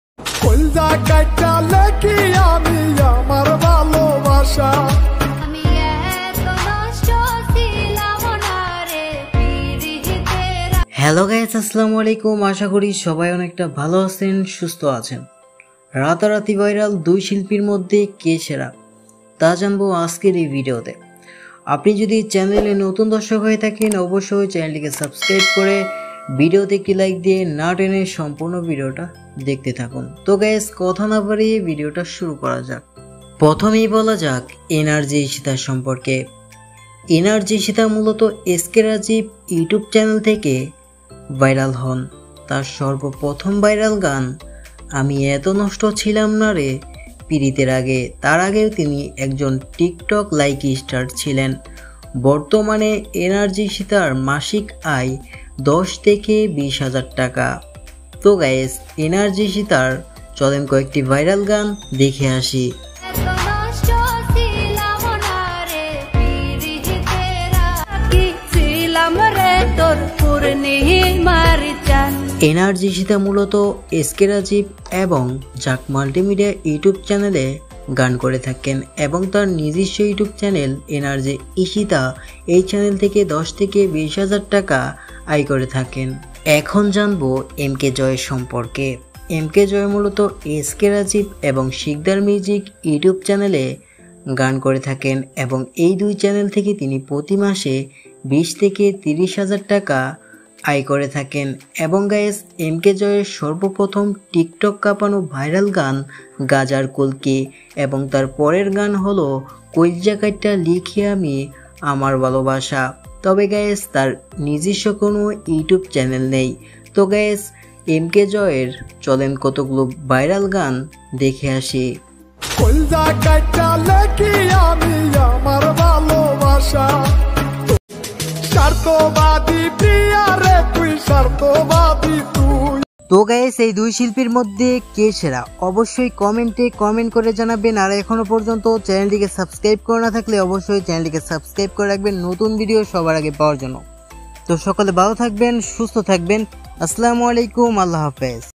Mk joy vs nrz eshita | রাতারাতি ভাইরাল দুই শিল্পীর মধ্যে কে সেরা | Mk joy Nrz eshita sad song
mk joy vs nrz eshita কে সের কত টাকা আয় করে কে ধনী বেশি

mk joy lifestyle mk joy song mk joy new song mk joy new song 2022 mk joy new sad  song